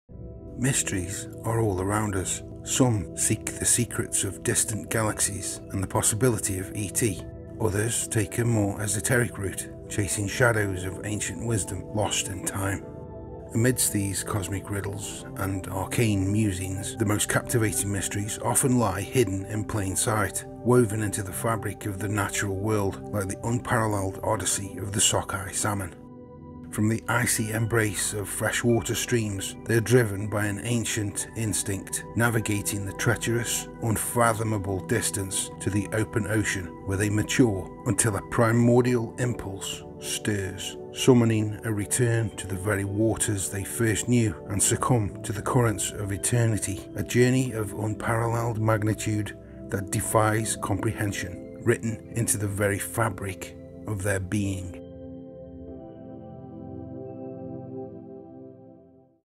Sockeye Salmon... A Primordial And Mystical Ebb & Flow...
In the Pacific Northwest, a spectacular journey unfolds every year—a journey that spans vast distances and defies the elements, the annual migration of the sockeye salmon; an enduring mystery of the natural world.
From the frigid waters of Alaska to the winding rivers of the Pacific Northwest, millions of sockeye salmon embark on a pilgrimage of epic proportions. Born in freshwater streams and rivers, these resilient creatures venture forth into the boundless expanse of the ocean, where they spend years roaming the depths in search of sustenance and sanctuary.
But as the seasons change and ancient instincts stir within their hearts, the salmon heed a mysterious call—a call that beckons them back to the very waters that first cradled their existence. And so, with a determination that borders on the miraculous, they begin their homeward journey, navigating treacherous currents and unforgiving terrain with uncanny precision.
It is a journey fraught with peril, yet imbued with a sense of purpose that transcends the boundaries of comprehension. From the tumultuous waters of the open ocean to the labyrinthine rivers that wind through dense forests and rugged mountains, the salmon press onward, driven by an inexorable force that guides their every movement.
And then, at long last, they breach the shores of their ancestral spawning grounds—a triumphant return to the place where life began. Here, amidst the rushing waters and ancient forests, they engage in a sacred ritual—a ritual of renewal and rebirth, of sacrifice and renewal.
In a frenzy of instinctual fervour, the salmon spawn, laying the foundation for future generations even as they surrender to the inevitability of their own mortality. It is a sight to behold—a symphony of life and death, of creation and dissolution, unfolding with solemn grace against the backdrop of nature's grandeur.